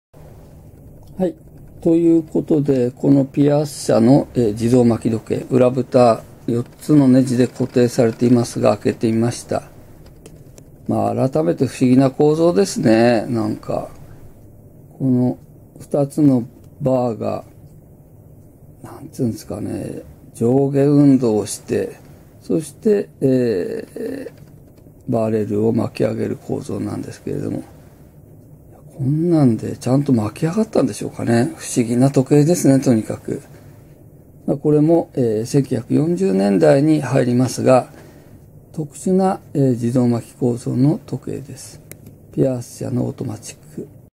PIERCEの自動巻き時計内部構造
Pierceは1883年スイスのビエンヌ（Biel/Bienne）でLeon Levyと彼の兄弟によって設立されました。正式名称は「Manufactures des Montres & Chronographes Pierce S.A.」。また同時期にスイス・ムティエ(Moutier)にも工場を構えます。社名の通りクロノグラフを中心に、当初より比較的廉価な時計をリリースしてきました。

　当時はクロノグラフ・ウォッチは一般的ではありませんでしたので、Pierceが製造するクロノグラフは非常によく売れ、同社はほどなく1500人もの従業員を雇うほどの大企業に成長しました。

　20世紀初頭、スイスの時計産業は過剰生産とメーカー間の価格競争によって恐慌状態に陥ります。1930年代には時計機械専業メーカーの倒産などから、完成品メーカーへの時計機械供給もままならなくなり、Pierceは自社キャリバーの開発を決断　特殊な機械構造を持ち1933年にパテントを取得した自動巻時計に搭載される時計機械も、他に例を見ない特殊な構造を持っています。「roller automatic」と呼ばれる同機は1940年に開発され、1954年まで製造が続けられました。